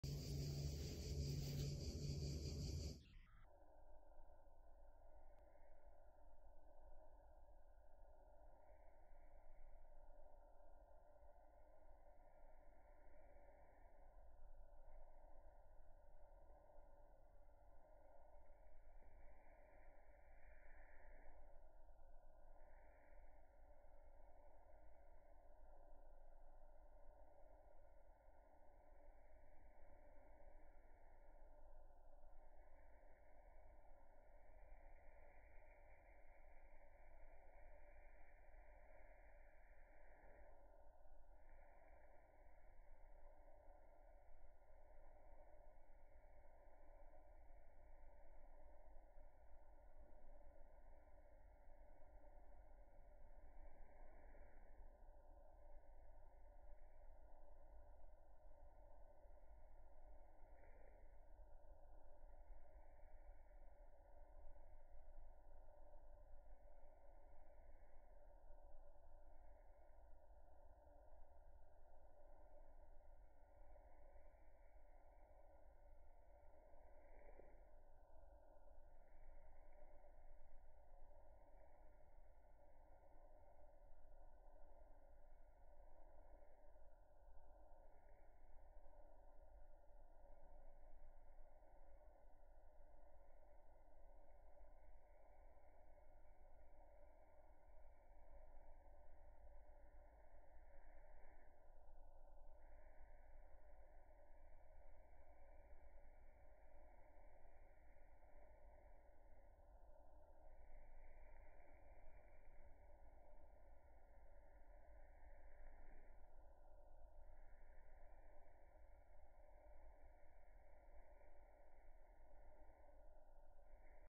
Artist painter Dimitri Likissas in the studio. Damien Hirst Spot Paintings on steroids.
Artist painter Dimitri Likissas in the studio varnishing a painting.
The painting being varnished is Liz Taylor in Dimitri Likissas signature dot style.